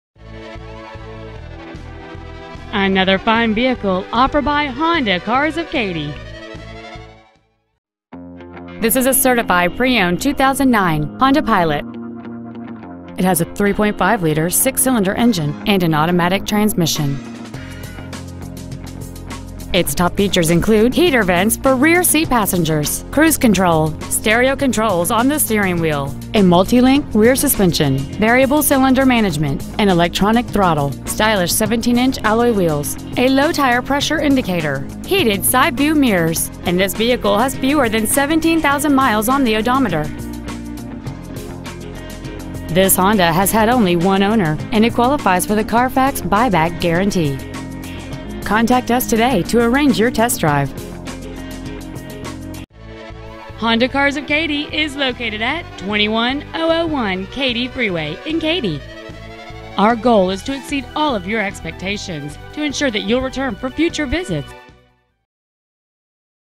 Certified 2009 Honda Pilot Katy TX
Year: 2009
 Make: Honda
 Model: Pilot
 Engine: 3.5L SOHC MPFI 24-valve i-VTEC V6 engine
 Trans.: Automatic
 Exterior: Burgundy
 Miles: 16,821
 Interior: Black

 2009 Honda Pilot Katy TX